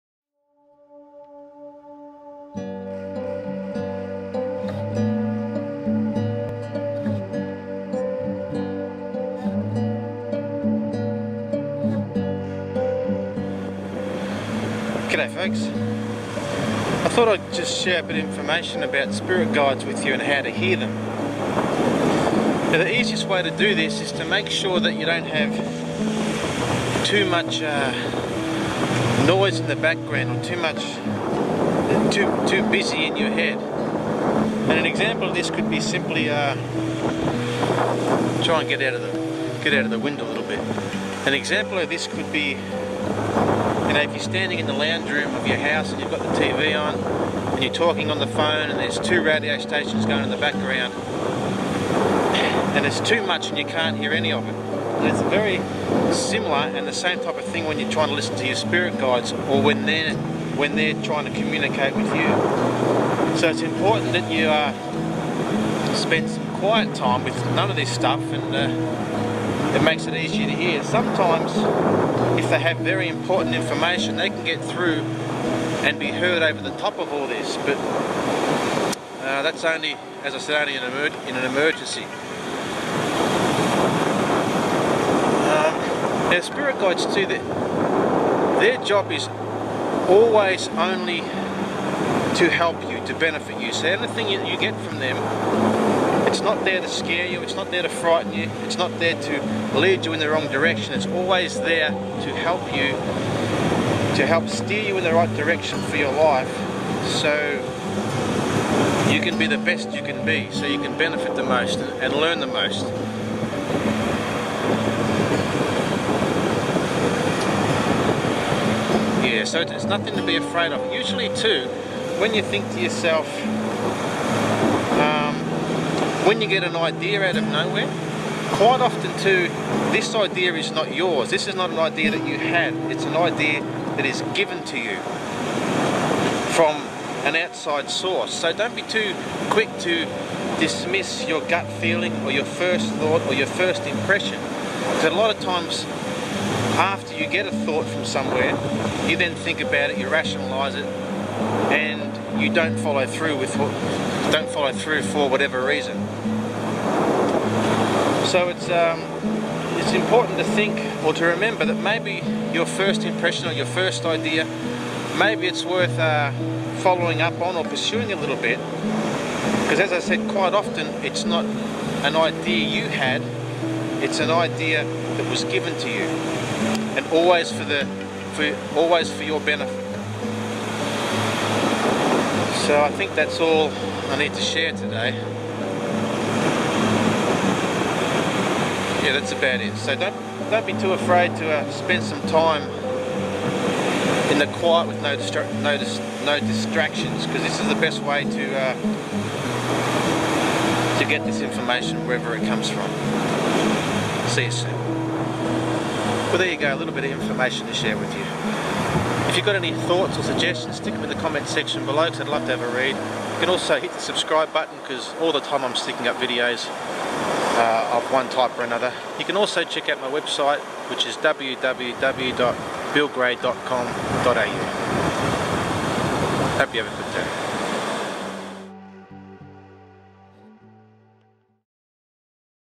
Spirit Guides : And how to hear them.
How to hear your Spirit Guides, and why you should not be afraid of them. In this Video Bill gives you some easy to use ideas and helpful hints.